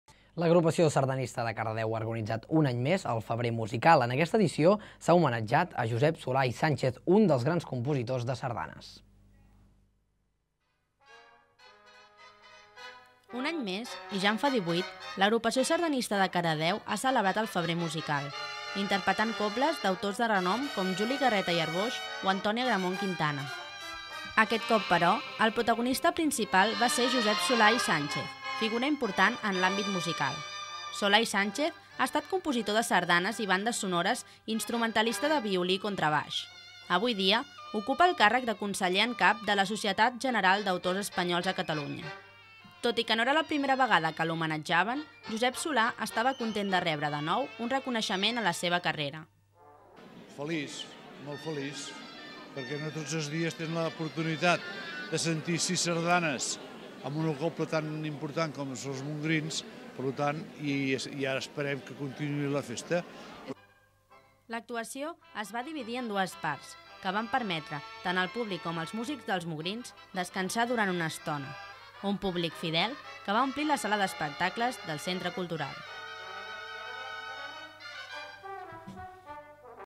04 El Febrer Musical homenatja Josep Solà i Sànchez
lAgrupació Sardanista de Cardedeu ha organitzat un any més el Febrer musical. En aquesta edició sha homenetjat a Josep Solà i Sánchez. un gran compositor de sardanes.